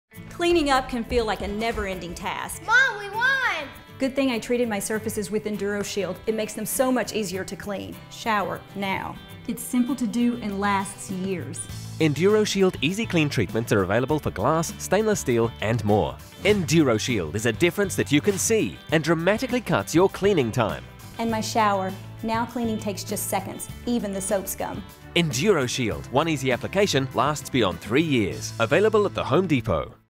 USA - EnduroShield Home TV Commercial
See our latest TV commercial for the New EnduroShield Home which is available at all Home Depot stores.